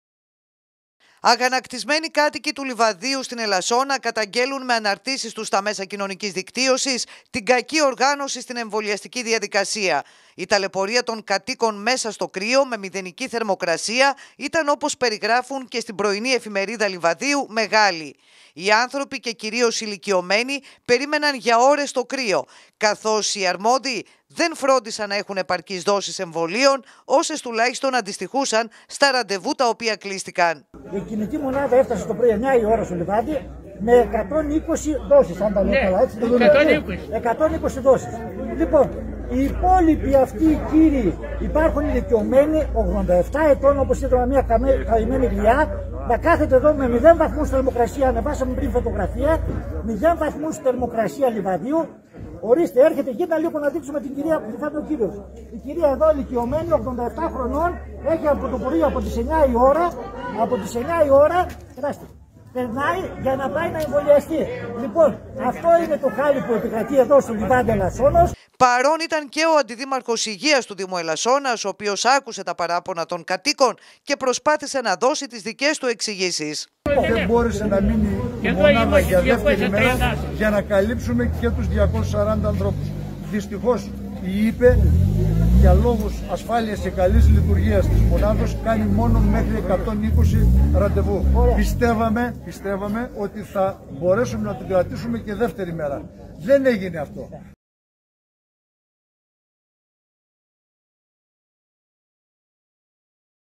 Λάρισα 239 άτομα περίμεναν να εμβολιαστούν. Οι δόσεις ήταν 120 131221
Το γύρο του διαδικτύου κάνουν απο το πρωι αναρτήσεις κατοικων από το Λιβάδι Ελασσόνας που καταγγέλουν κακή οργάνωση της εμβολιαστικής διαδικασίας .
Οπως λενε ενω κλείστηκαν 239 ραντεβου οι δοσεις  που ειχε μαζι της η κινητή μονάδα ηταν μόλις 120 και πολίτες ταλαιπωρήθηκαν περιμενοντας για ώρες στο κρύο.